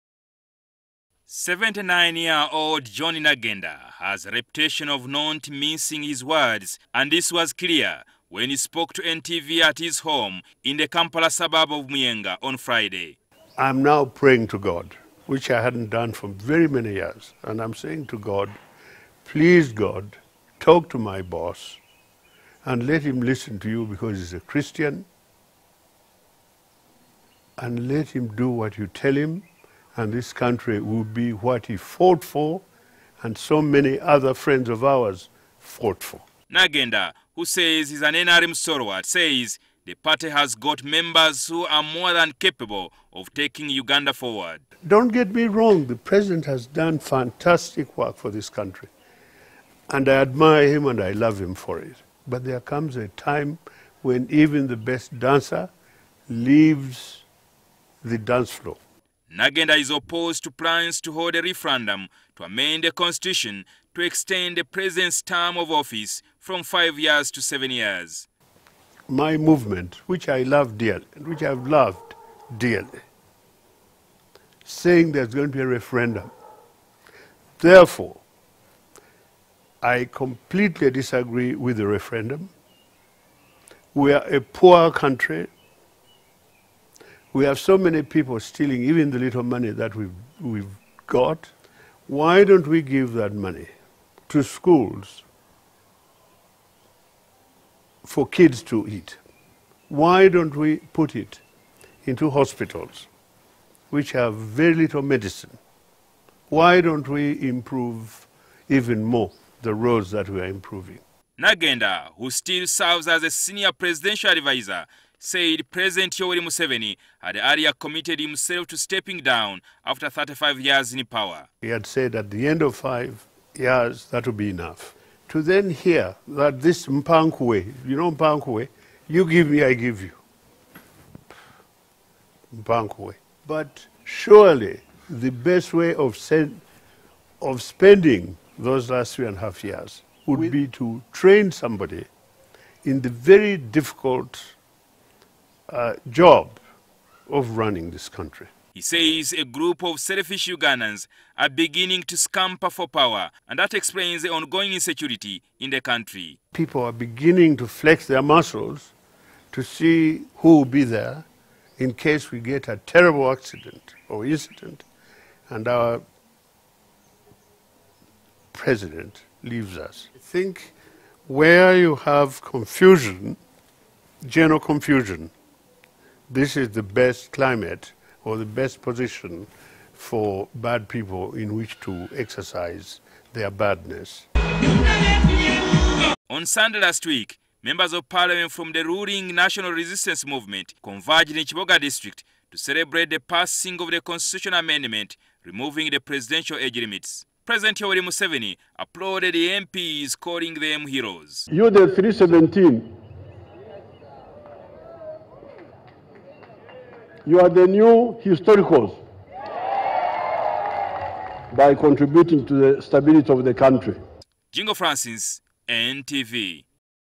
Nagenda advises President Museveni to groom successor
Senior Presidential Adviser on Media and Public Relations, John Nagenda, has asked God to intercede and restrain Yoweri Museveni from extending his term of office as president.

In an interview with NTV, Nagenda says he hopes President Museveni will instead groom a successor

Nagenda, who says he is still an NRM supporter, has also cautioned his party members to stop agitating for a referendum to extend the term of the president from five to seven years.